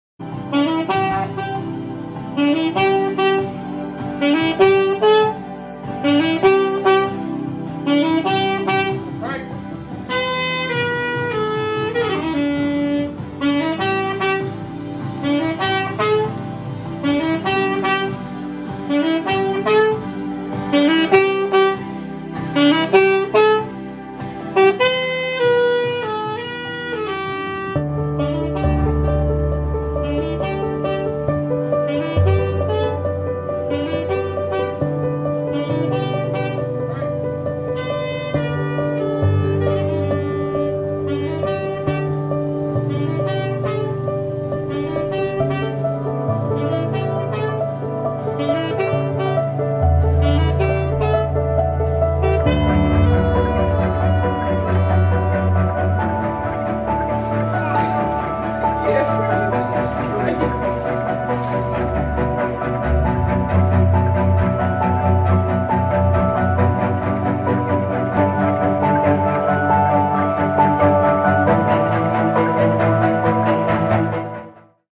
This baby is mesmerized by his grandpa's saxophone playing skills
Grandpa Joe Sims has been playing the saxophone for baby Braylon since he was born.